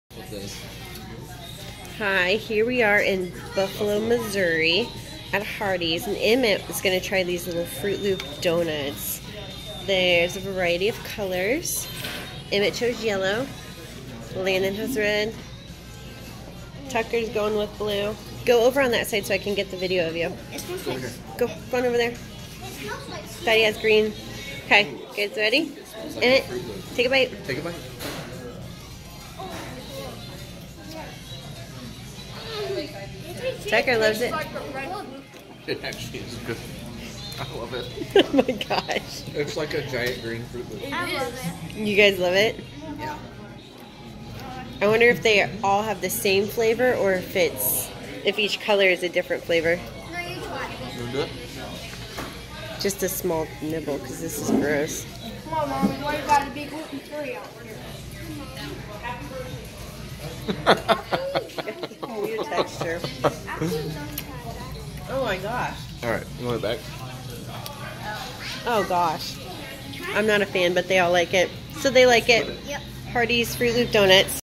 Taste test challenge: Hardee's Froot Loops Donuts
We are out on vacation in Missouri and thought we'd try these new Froot Loops mini donuts. (I personally don't like them.)